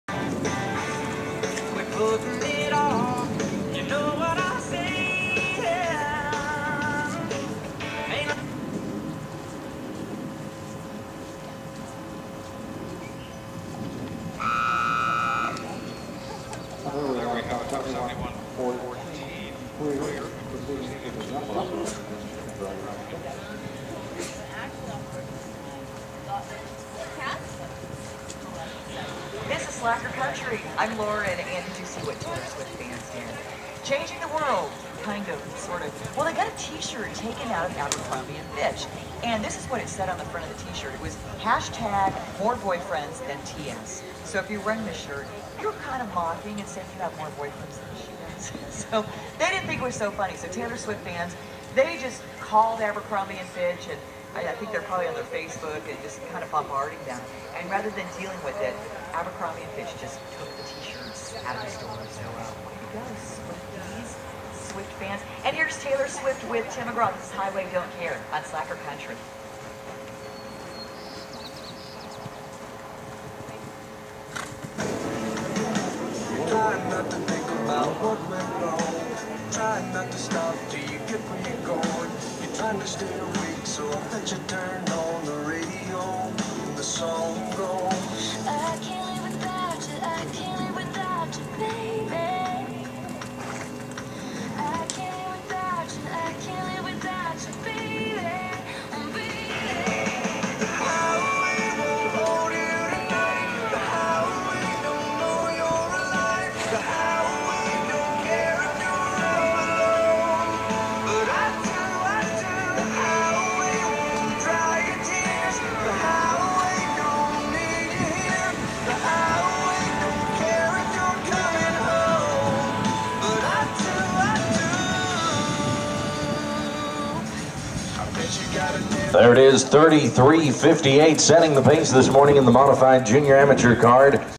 Burlesque ridden by Kristen Senger
Thunderbird Show Park 2013
Modified Jr/Am Jumper